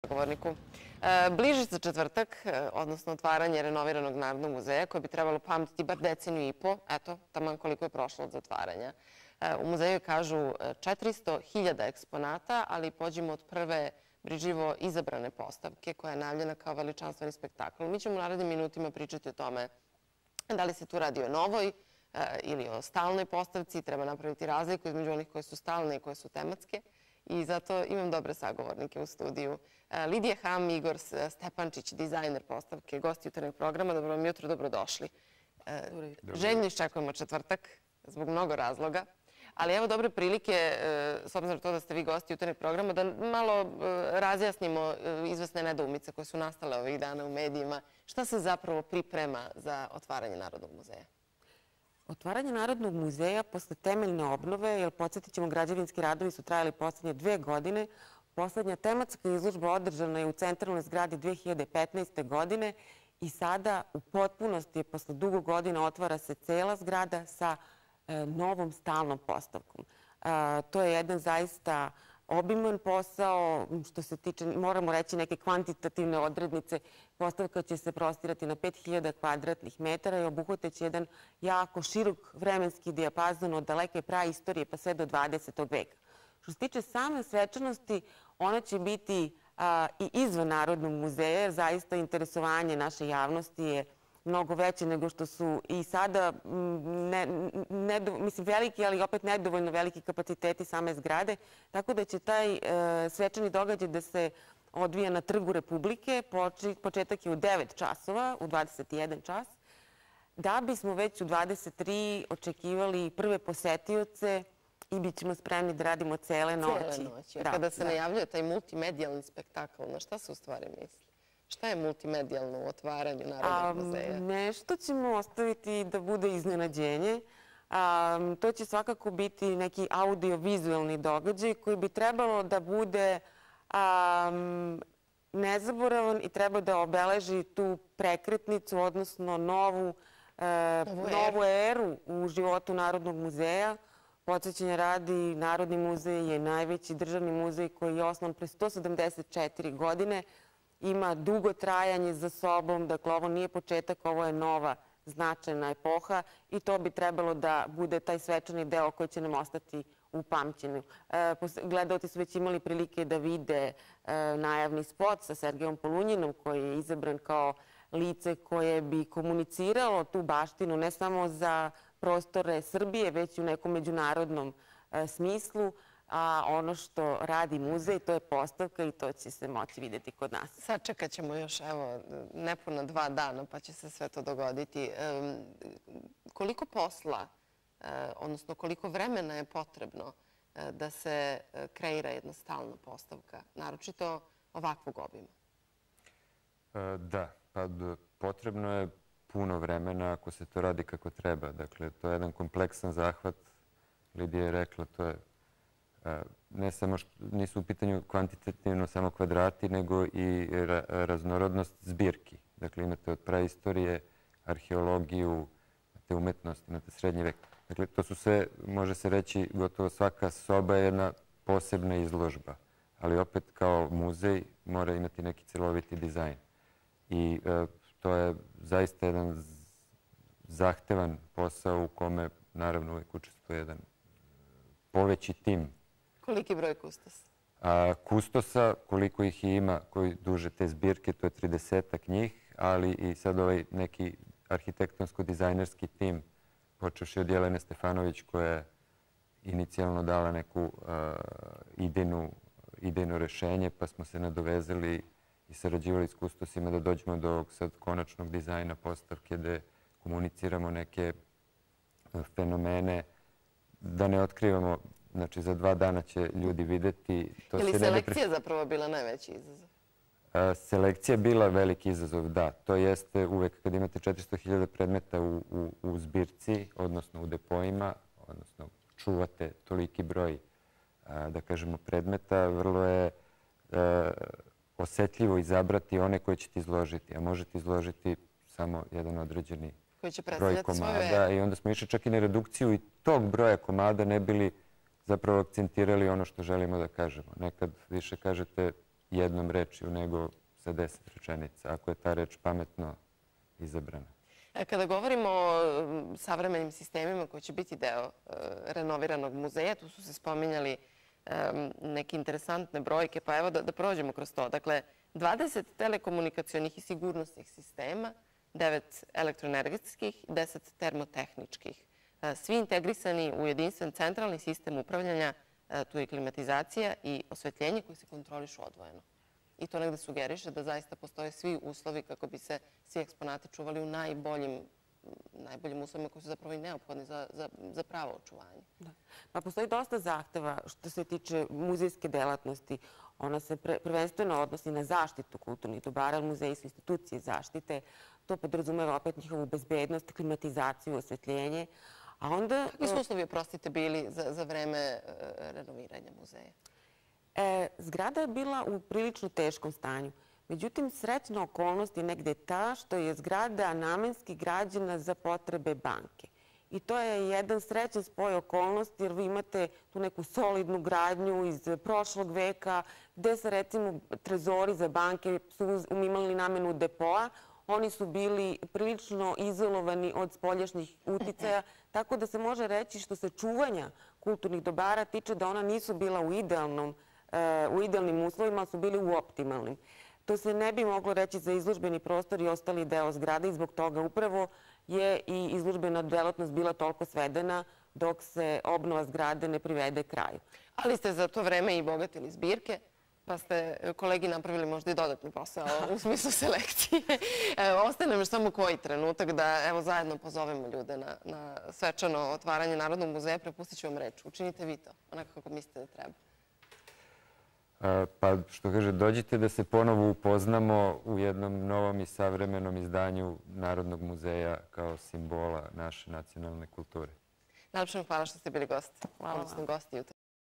Pred otvaranje
Bliži se otvaranje renoviranog Narodnog muzeja. Sa nama su Lidija Ham i Igor Stefančić, dizajner postavke.